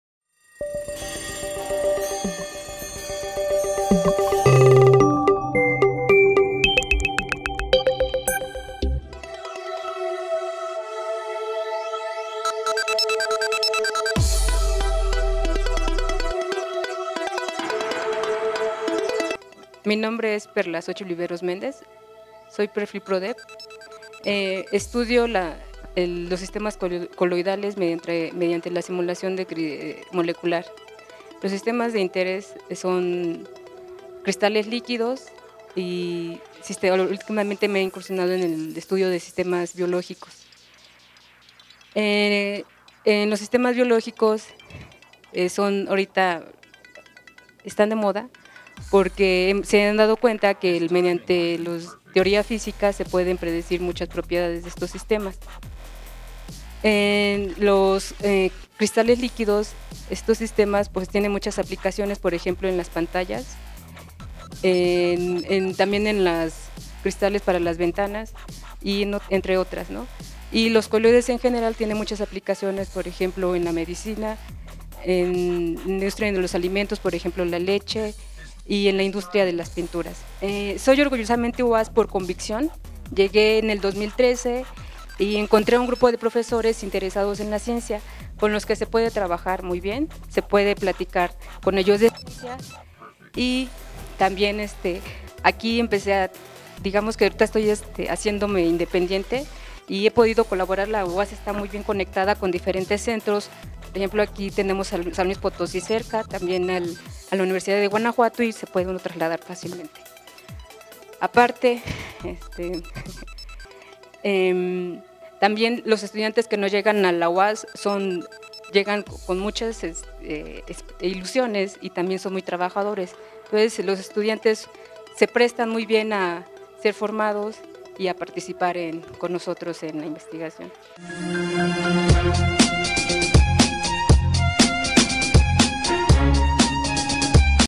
Investigadores UAZ
¿Qué se investiga en la UAZ?
Perla Xochil Viveros Méndez
Docente Investigador UA de Física